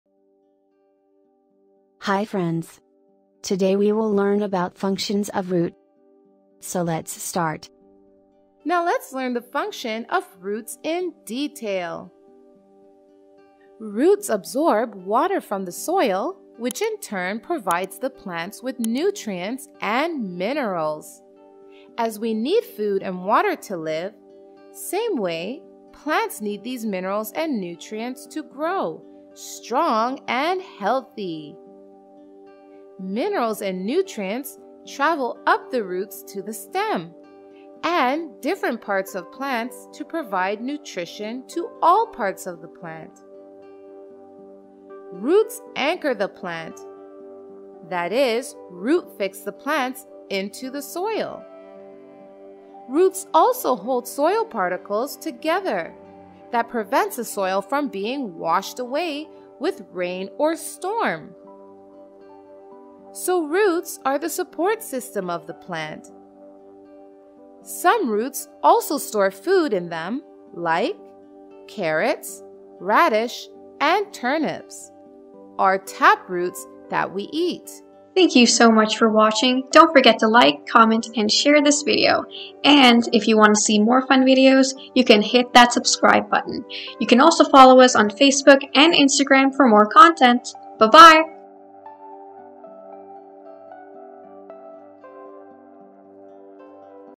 Functions of Roots
Functions of Root in Plants | Root System - Roots, Types of Roots and Functions of Roots | Science
This educational video explains about roots. This will help them a lot in their daily routine.
Roots
Types of Roots
Functions of roots in plants
Functions of root system